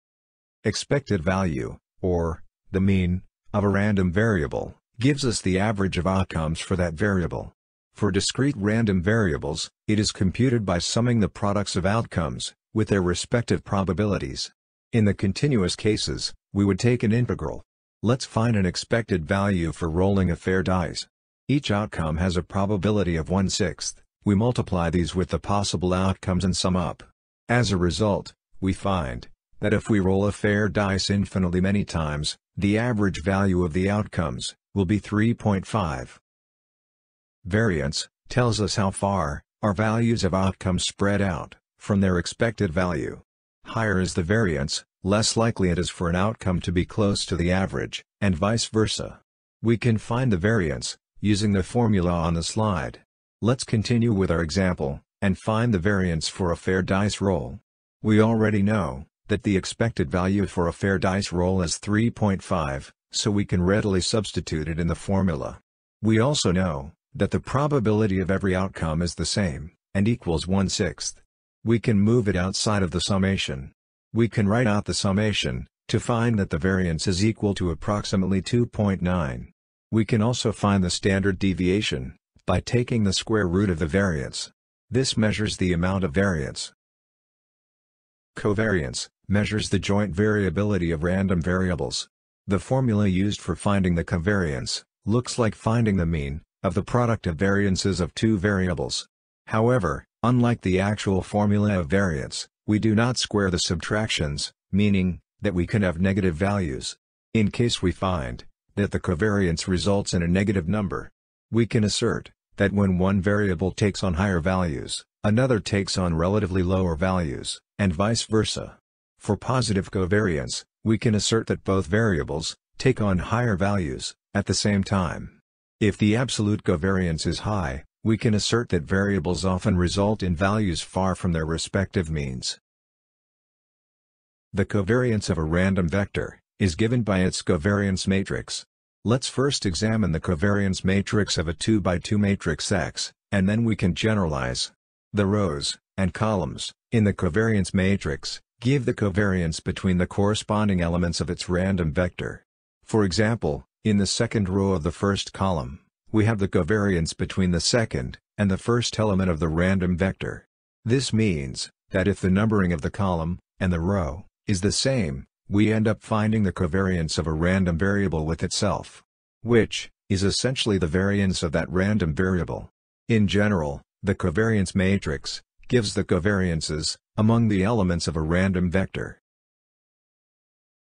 Expected Value, Variance, Covariance, Covariance Matrix
0:00 - Expected Value
0:35 - Variance
1:26 - Covariance
2:13 - Covariance matrix

Recommended reading: Deep Learning An MIT Press book Ian Goodfellow and Yoshua Bengio and Aaron Courville
Slides made in: Microsoft PowerPoint